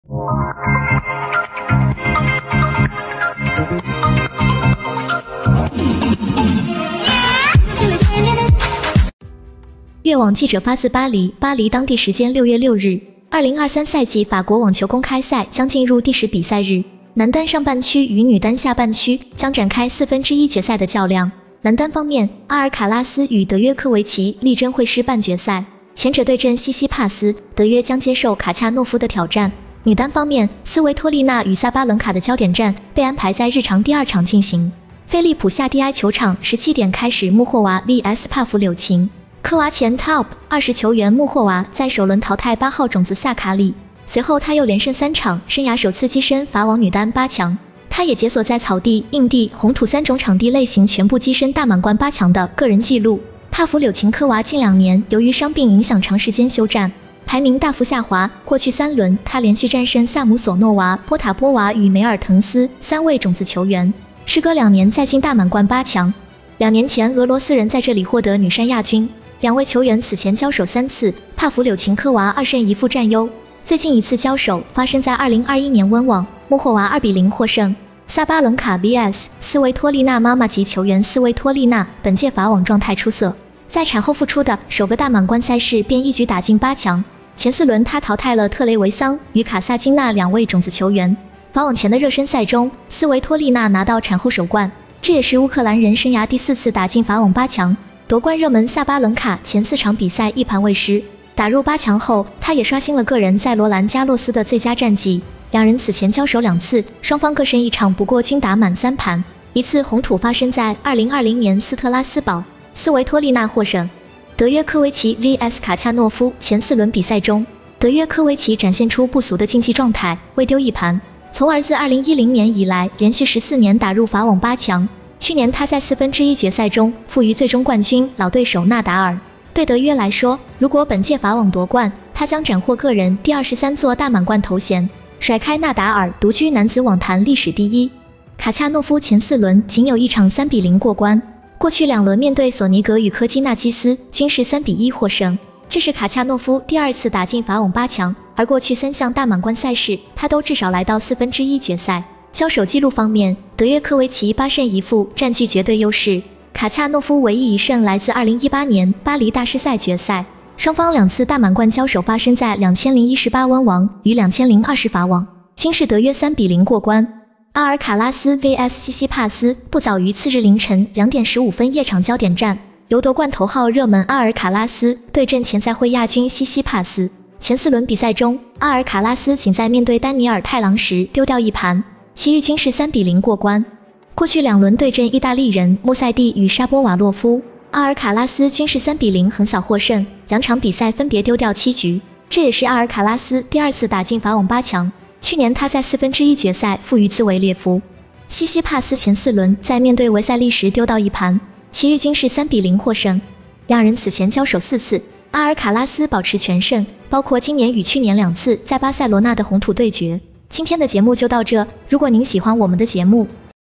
法網第10天：德約阿卡盼會師，薩巴倫卡斯維托麗娜貢獻火力對決
巴黎當地時間6月6日，2023賽季法國網球公開賽將進入第10比賽日。菲利普·夏蒂埃球場 17:00開始。